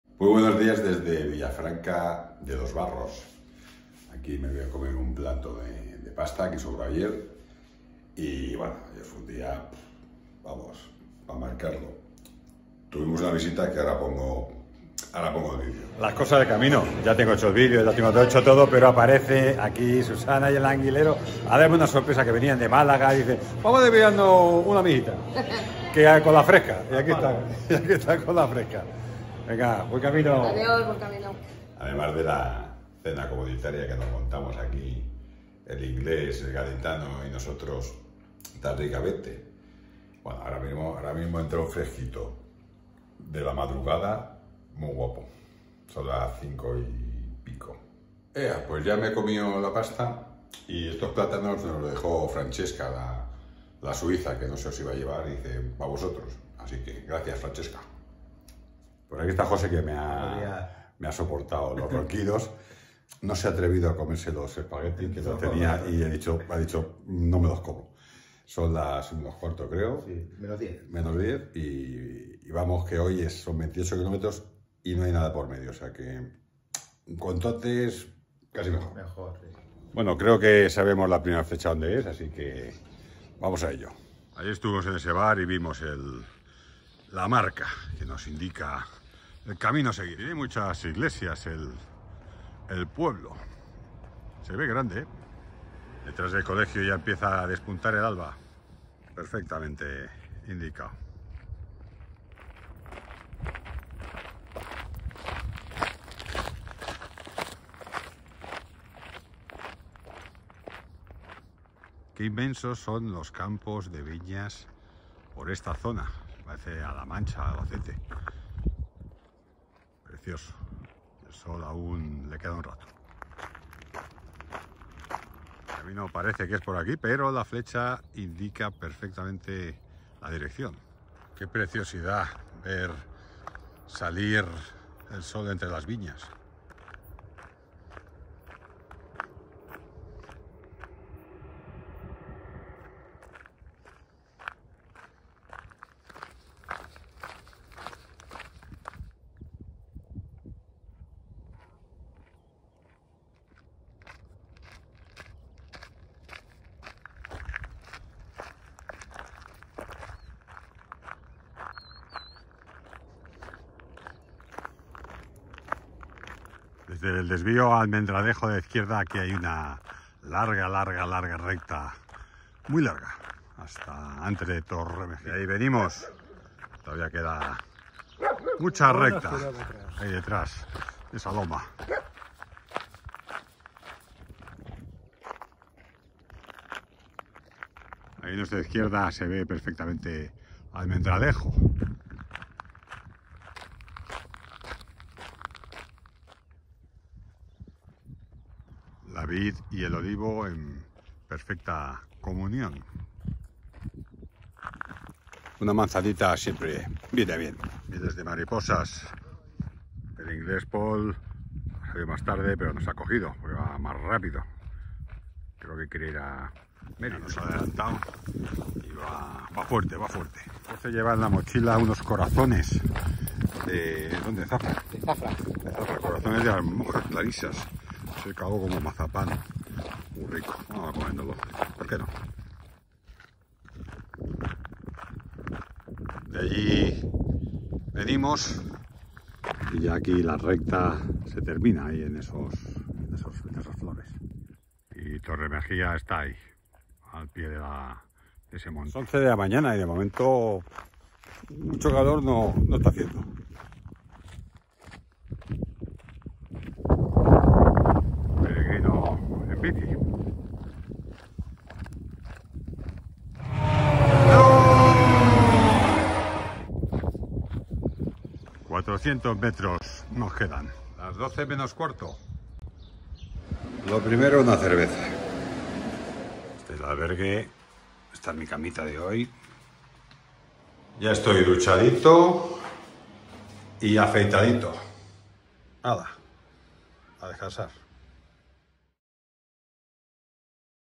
Camino 49 Vía de la Plata Junio 2022 Parte 2
De Villafranca de los Barros a Torremejía el 13 de junio de 2022…